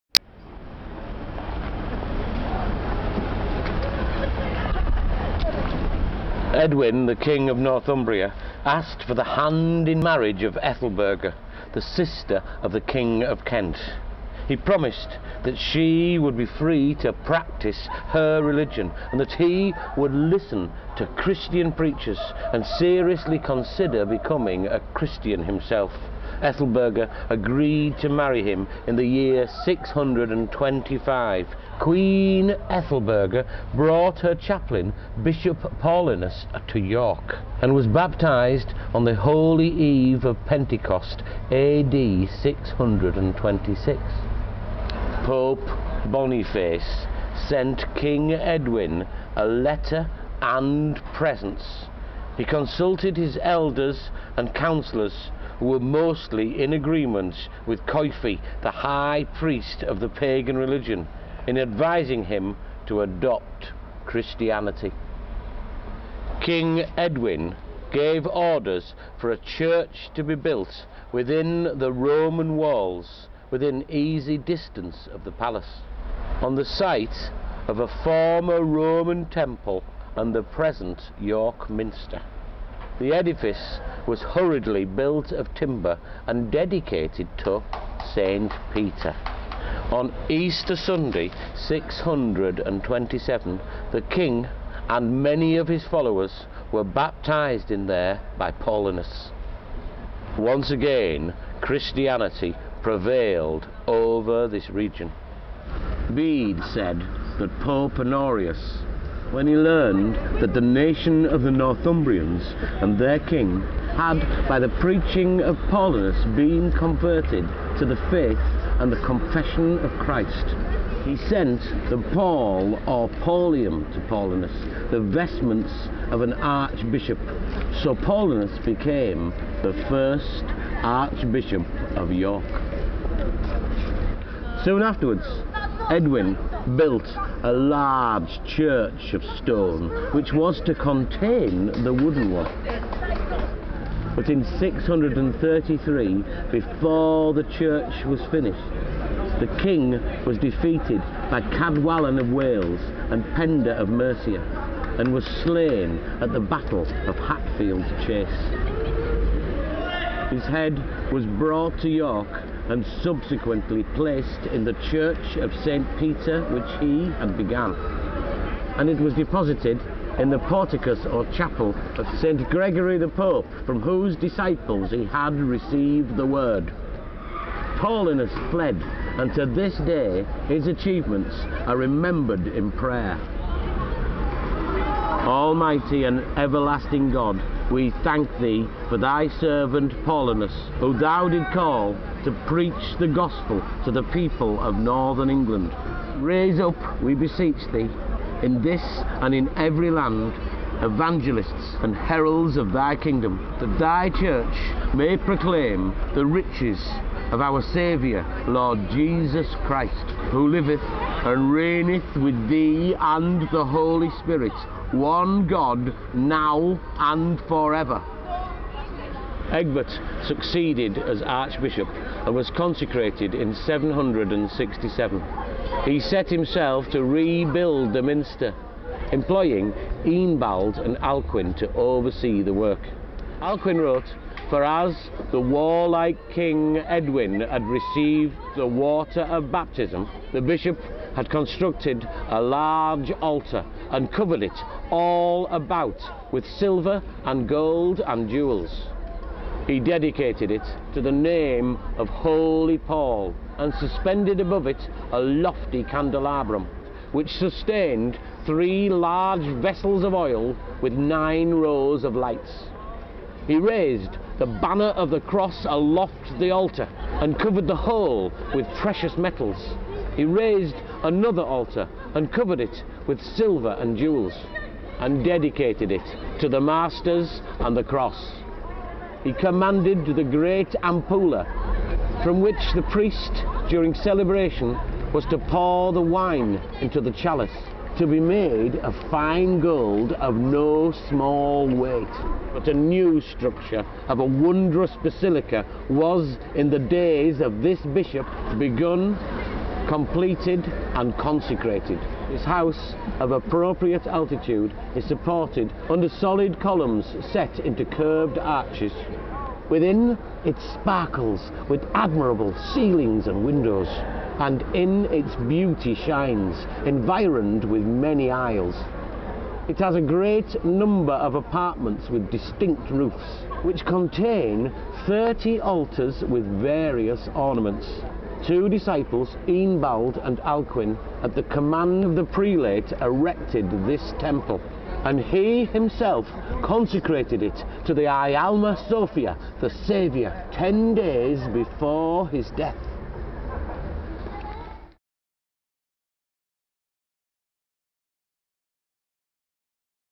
Adrian Spendlow reads from 'A history of York Minster'
Excerpts from the Spendlows York audiobook read by Adrian in front of The Minster during the Illumination festival